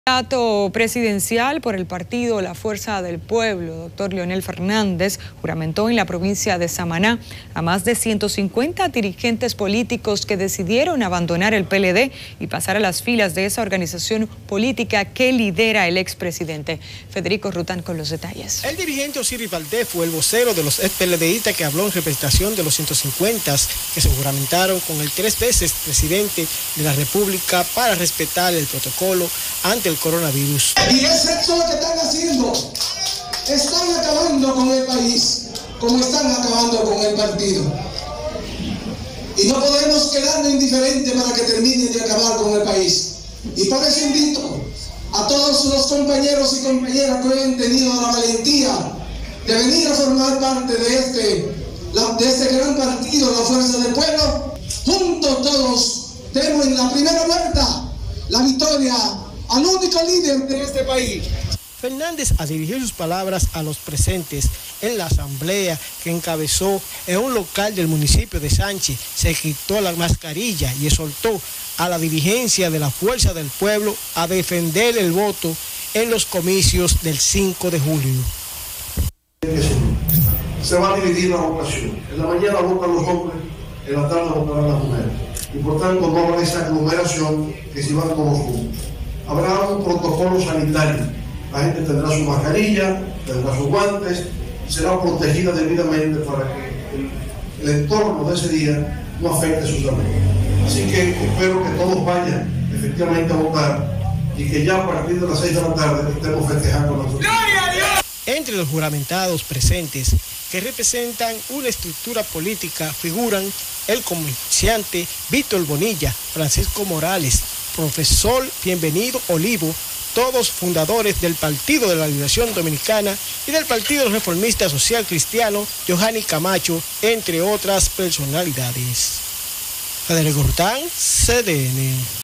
Leonel Fernández Juramenta en Samaná dirigentes que abandonaron el PLD
El candidato presidencial de la Fuerza del Pueblo, Leonel Fernández, juramentó en la provincia de Samaná a más de 150 dirigentes políticos que decidieron abandonar el PLD y pasar a las filas de esa organización política que lidera el expresidente.

El dirigente Osiris Valdez fue el vocero de los  expeledeístas que hablo en representación  de los 150 que se juramentaron con las tres veces presidente de la república,  para respetar  el protocolo ante el coronavirus.

Fernández, al dirigir sus palabras a los presentes en la asamblea que encabezó en un local del municipio de Sánchez, se quito la mascarilla y exhortó a  la dirigencia de la Fuerza del Pueblo a defender el voto en los comicios del  5 de julio.